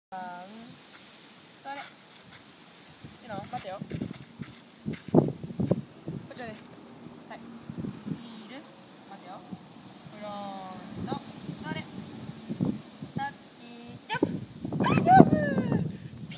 イタグレ junon 10か月 立っちでピョンピョン
2012/4/1生まれ
2013/2/21撮影
二足でジャンプの練習